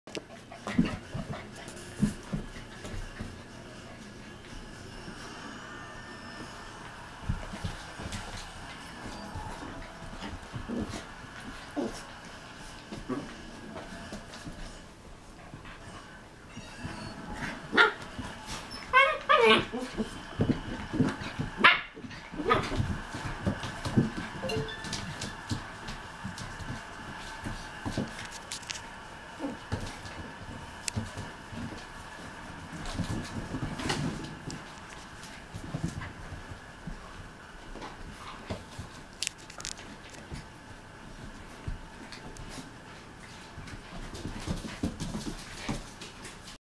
Australian Shepherd Puppies 7-1/2 weeks old wrestling aussies
Three aussies wrestling and having a good time together.